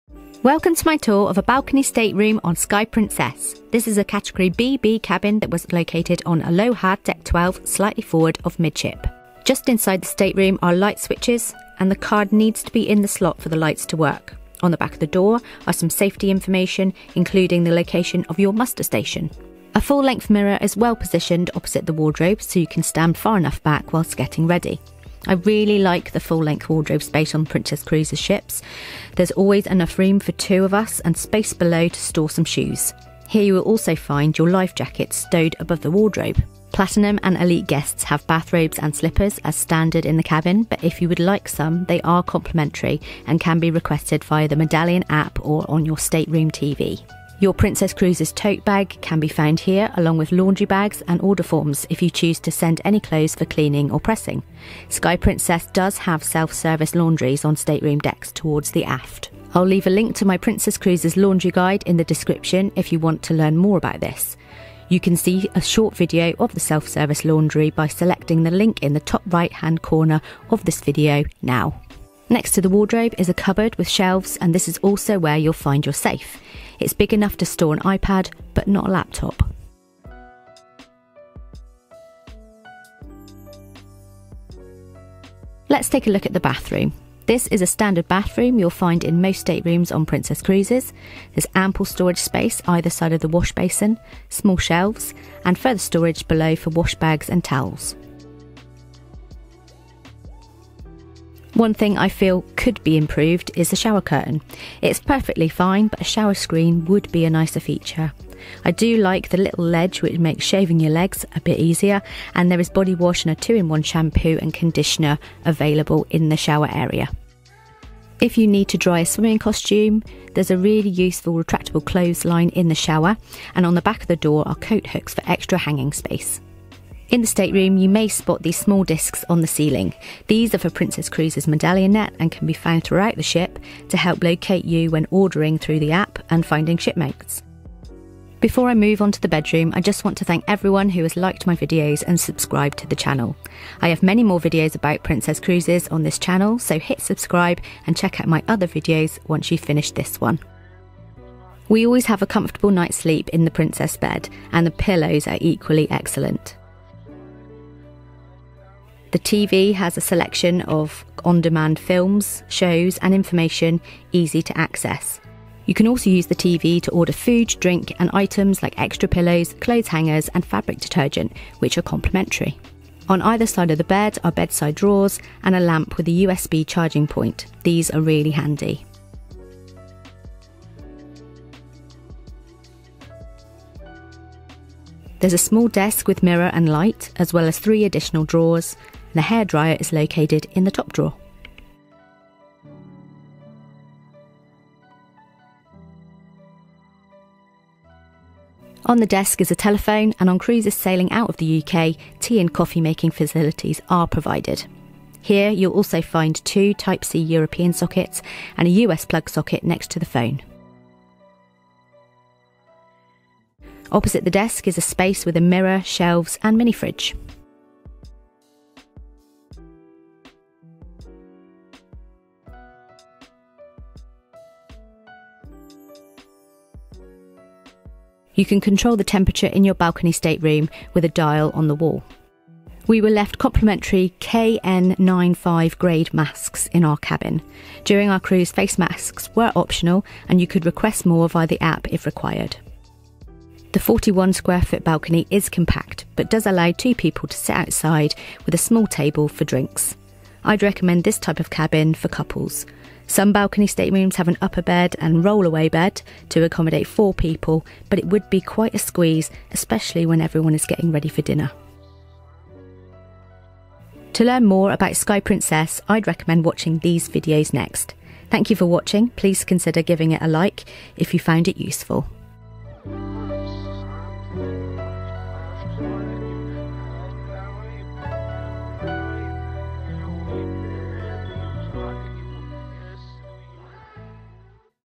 Sky Princess Balcony Cabin Tour
This is a tour of a balcony stateroom on Sky Princess. The cabin is category BB and was located midship on Deck 12 (Aloha).

The  balcony cabin is perfect for a couple and features a Princess Luxury Bed, large wardrobe area, mini-fridge and 41 square foot balcony.

For more details about the laundry services available to guest on Princess Cruises you can check out my laundry guide princess balcony cabins

sky princesscruiseshipcabins